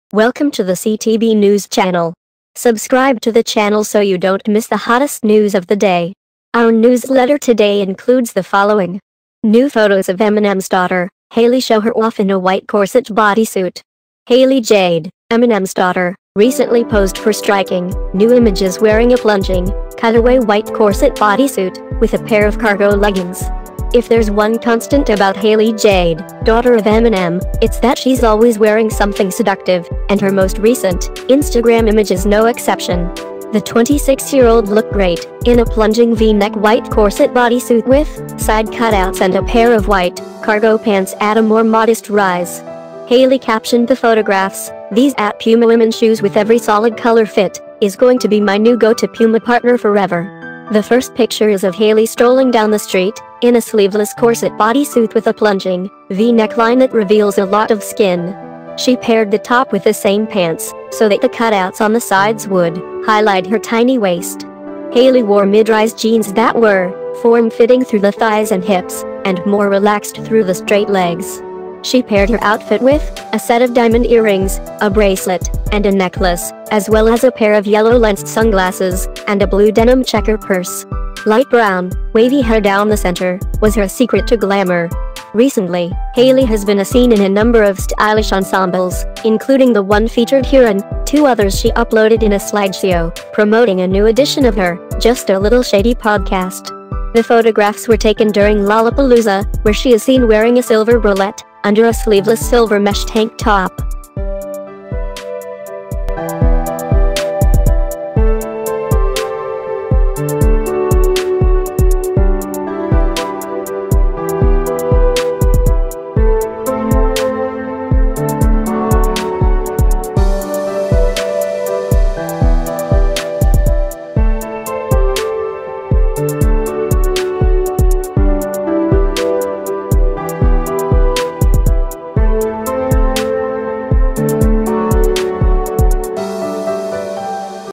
New photos of Eminem's daughter Hailie show her off in a white corset bodysuit | CTB NEWS
CTB News - New photos of Eminem's daughter, Hailie show her off in a white corset bodysuit.
Hailie Jade, Eminem's daughter, recently posed for striking, new images wearing a plunging, cutaway white corset bodysuit, with a pair of cargo leggings.